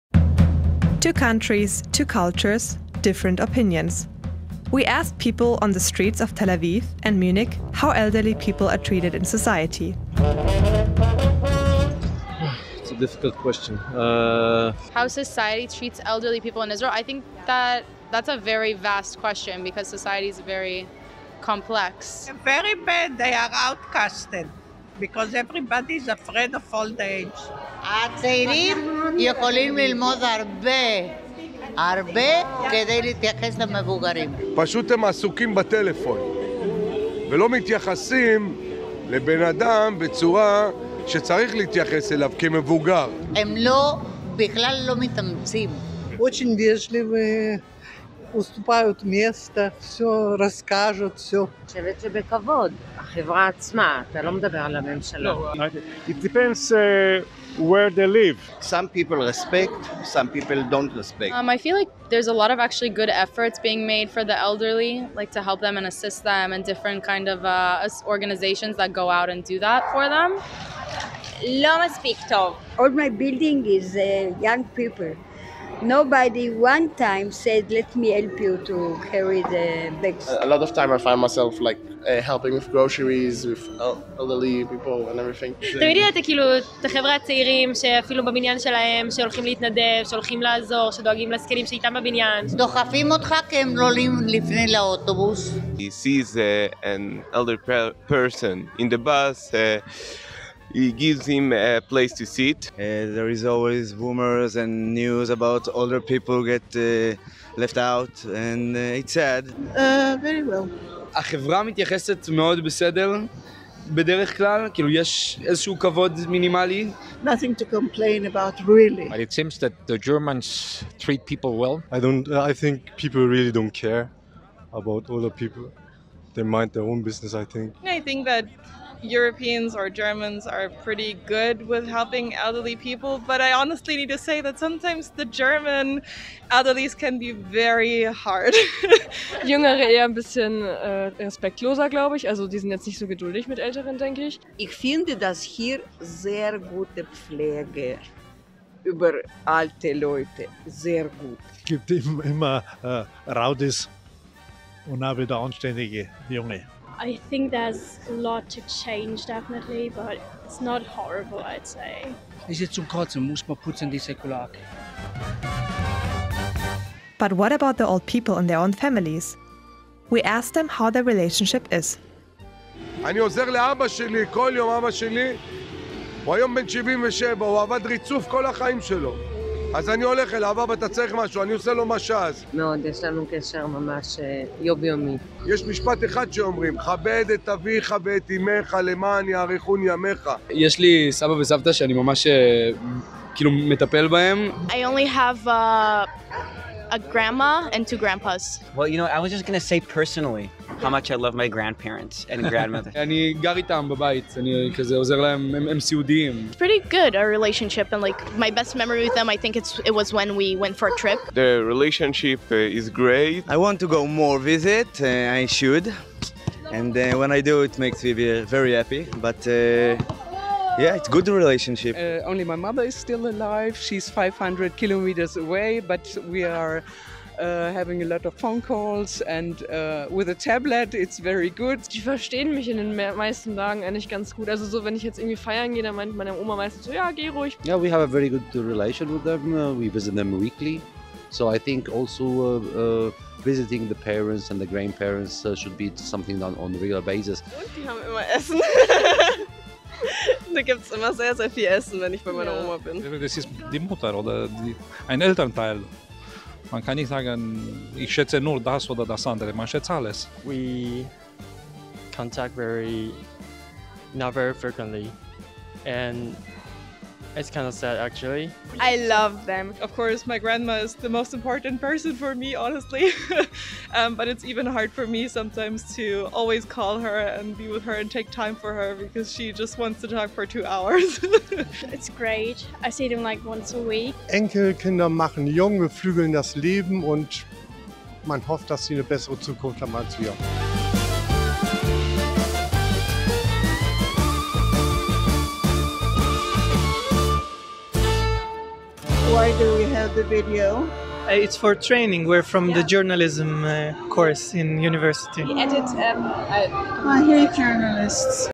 Israel vs. Germany: How Does Society Treat Elderly ? - Hanns Seidel Foundation Exchange 2022
How does society treat elderly? What do Germans think, what do Israelis think? Is there any difference at all in mentality? We checked on the streets and asked strangers for their opinions.

This video was produced by Sarah Zapf and Danel Shvetz during an exchange program from Hanns Seidel Foundation between Tel Aviv University and Deutsche Journalistenschule in 2022. The given topic was: "Age and Society's Attitude Towards Elderly".